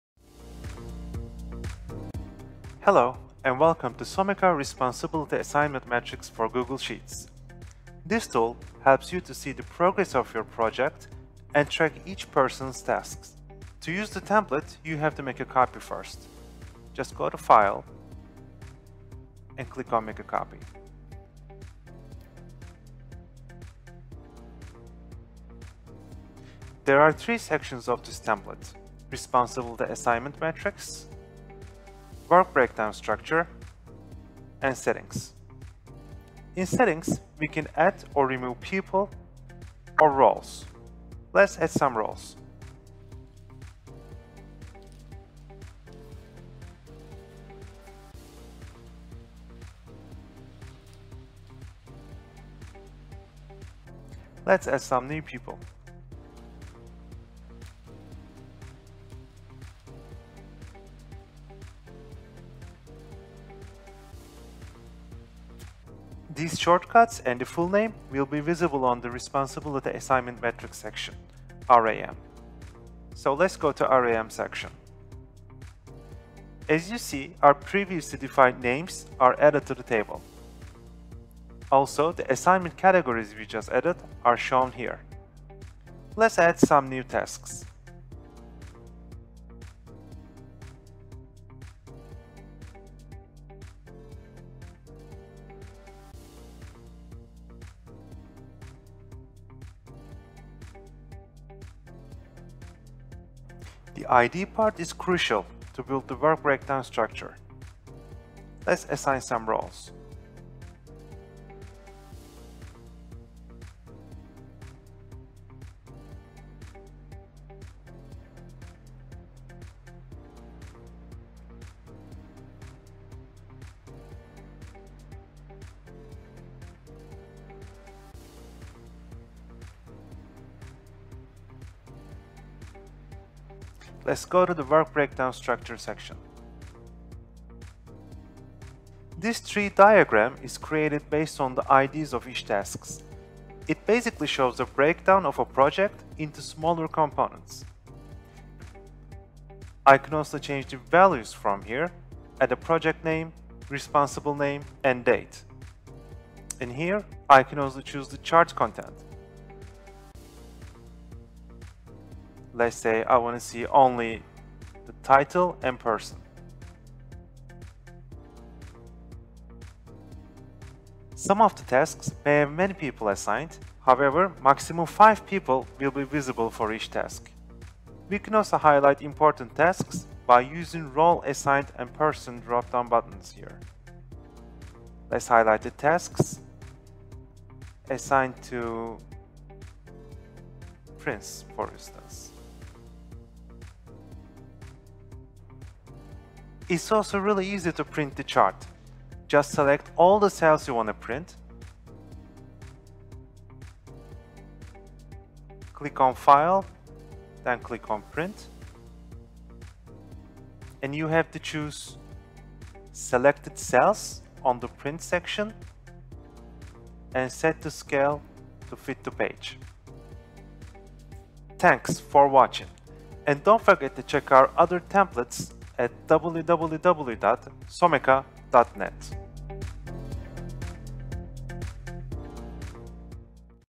Responsibility Assignment Matrix Google Sheets Template | RACI
You have lots of tasks with different teams of employees, and don't know how to assign tasks to particular people?

Here is the RACI Matrix (Responsibility Assignment Matrix) Google Sheets Template with Work Breakdown Structure.

Ready-to-use Google Sheets template. Access from all devices.

# Project management tool in Google Sheets
# Create, assign, and manage tasks & responsibilities